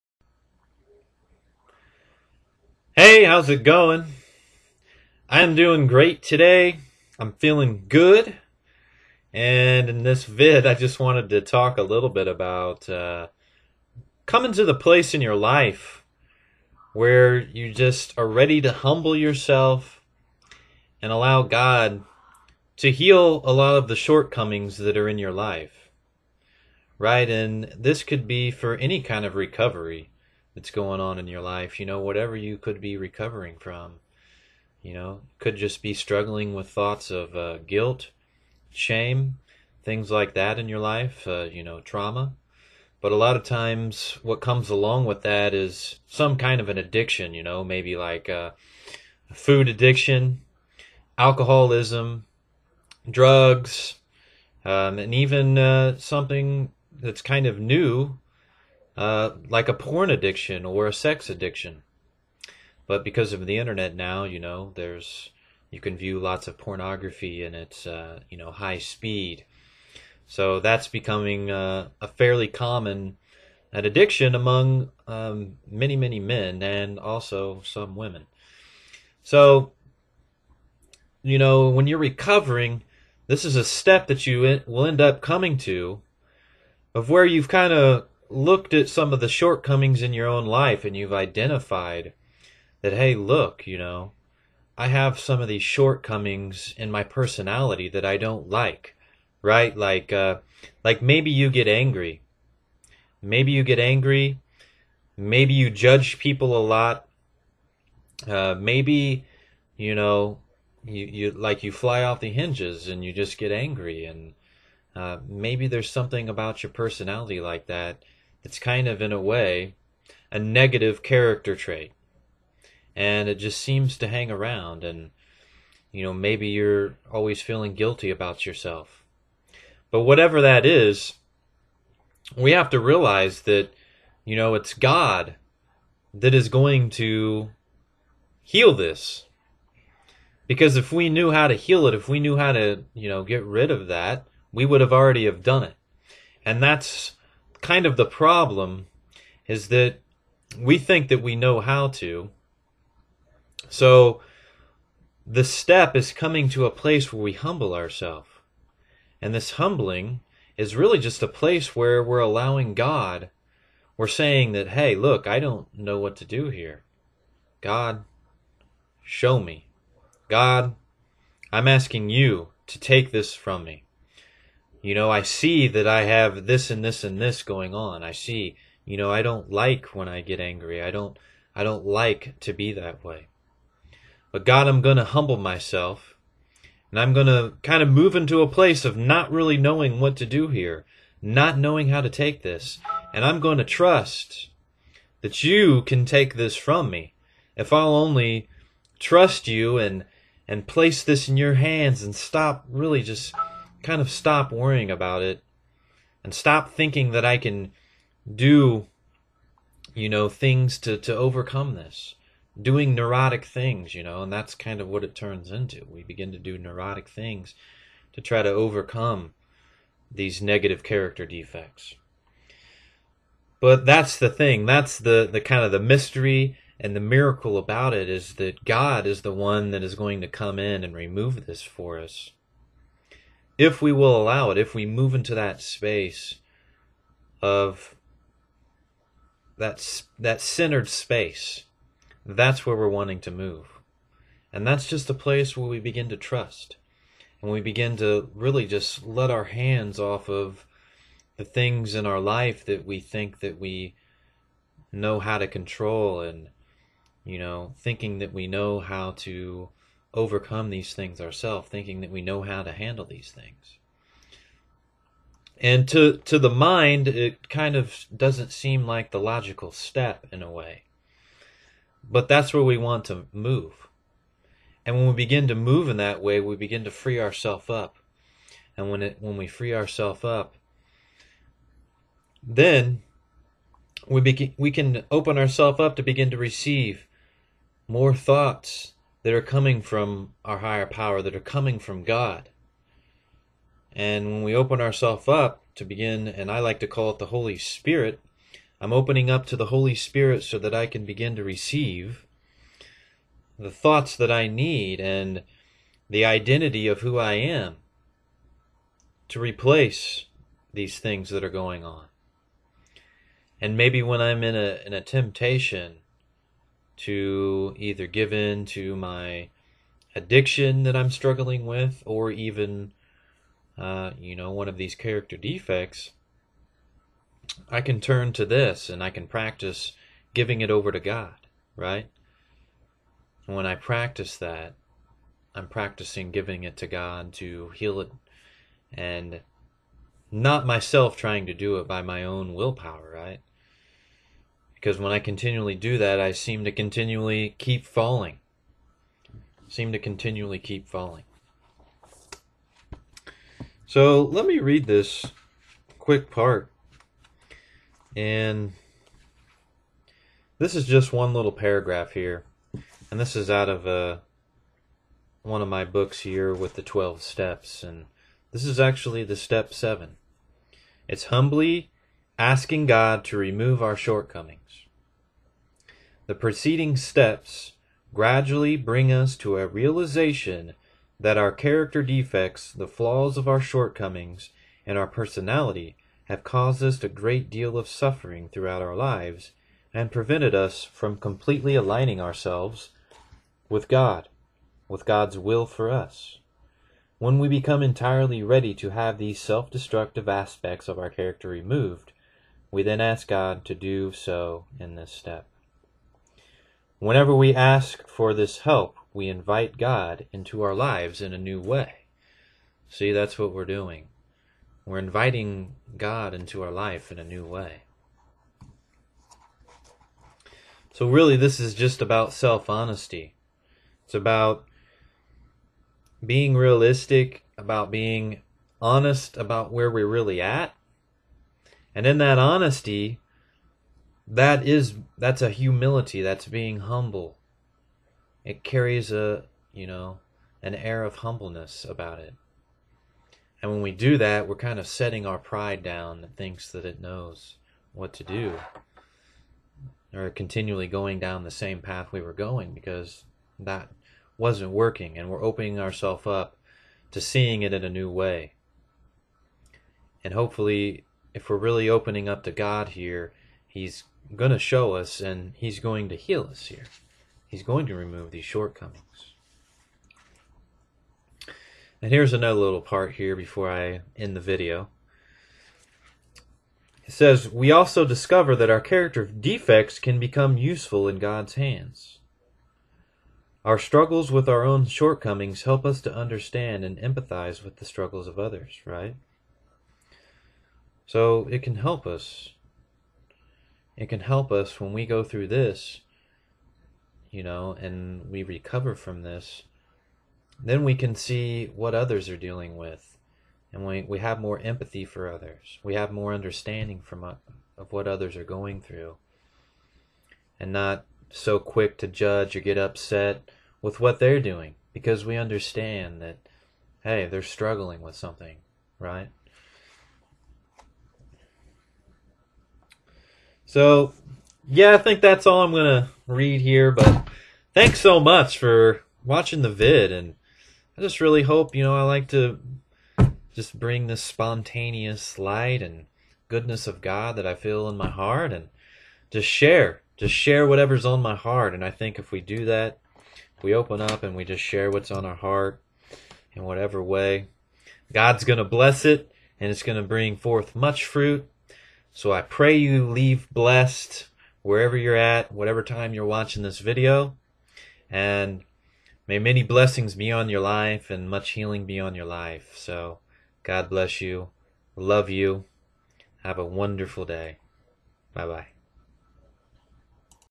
Happy Are The Humble
Healing happens in God's timing. In our self-honesty about our strengths and limitations we open our self to seeing our shortcomings in a different way. Our humbleness is an act of trusting God and showing willingness to place our faults in His hands. He is a big boy and He can take care of them. Trust me!

“Happy are those who are humble;
    they will receive what God has promised!
~Matthew 5:5